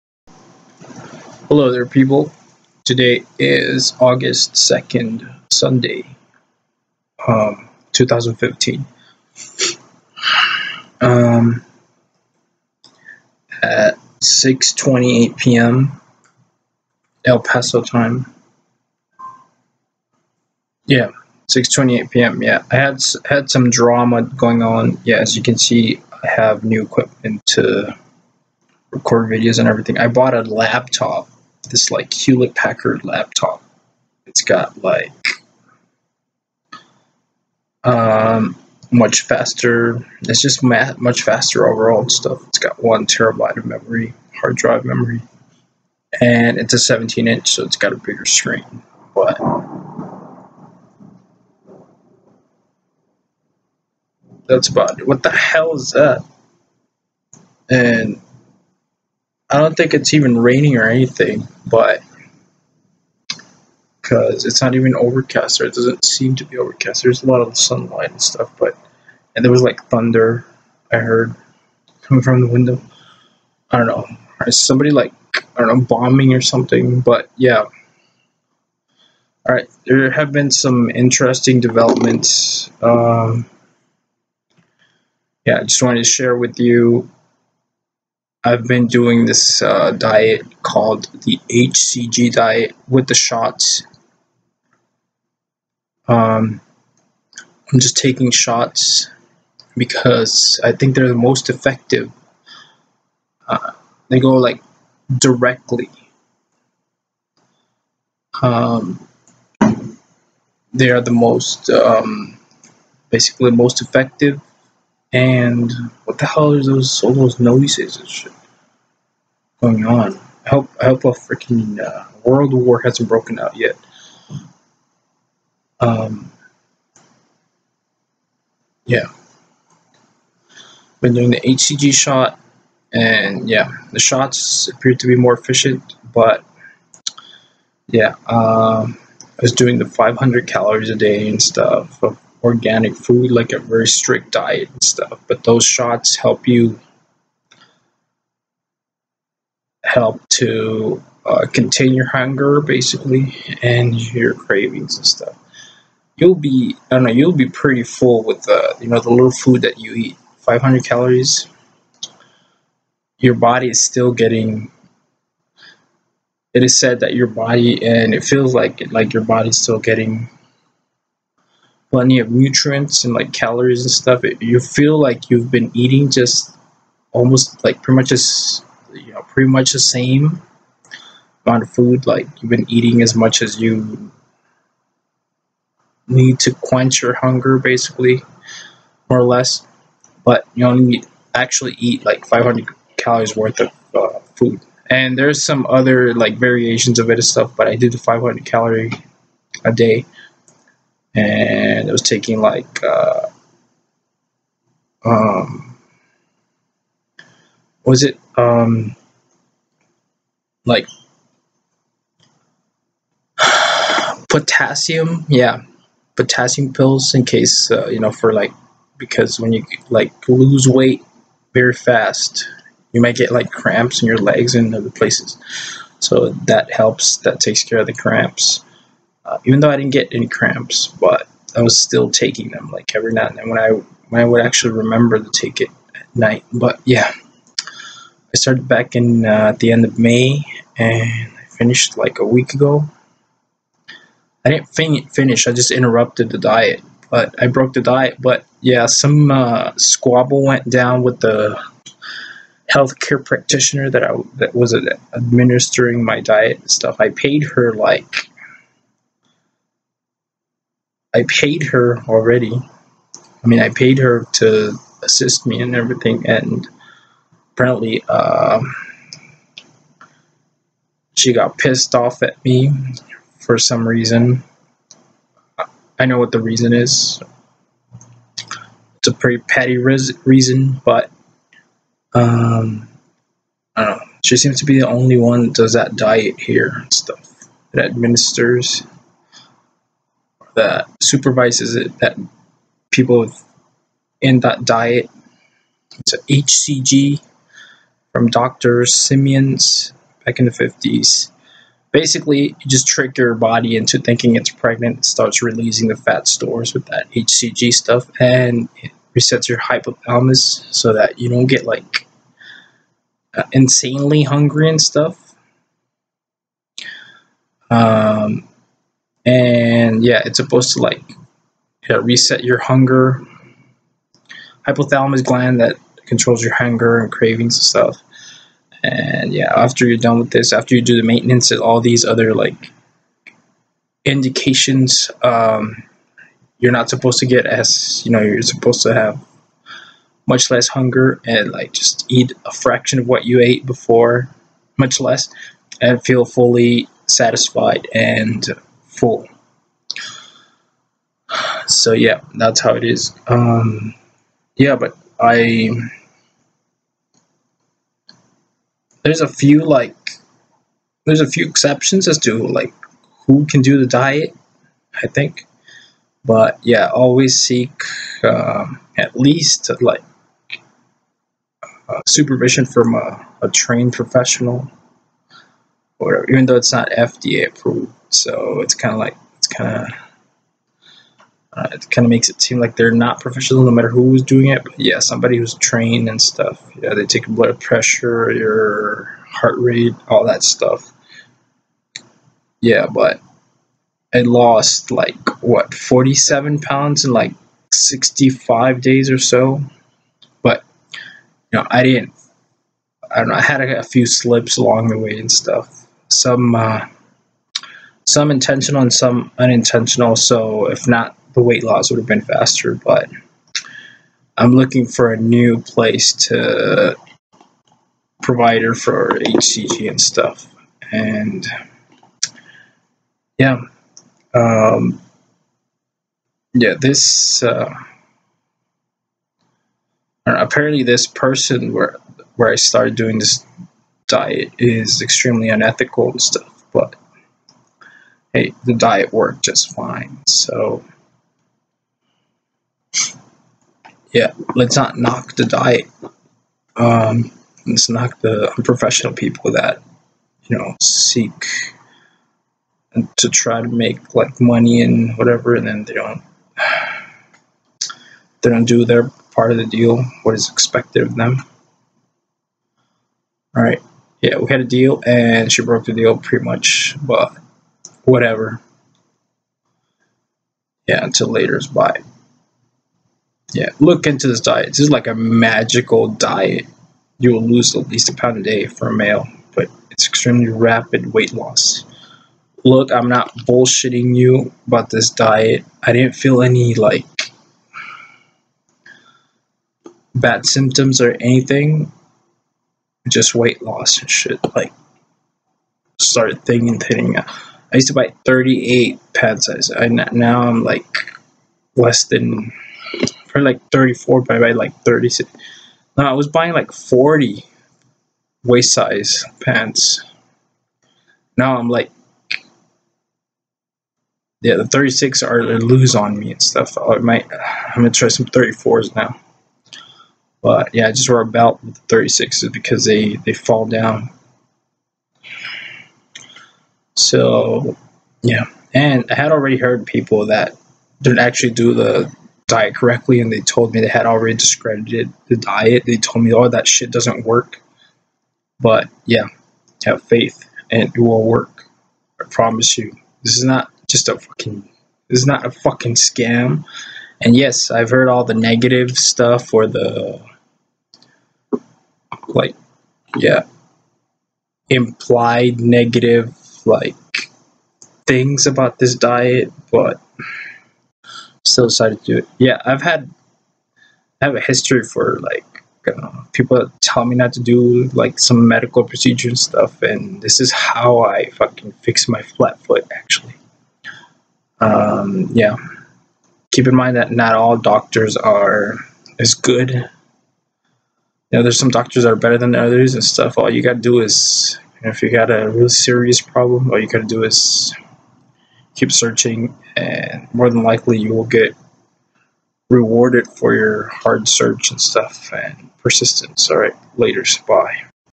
Success with the hcg diet but not with the healthcare professional administering it.
Hcg diet works ive been taking a break and eating tho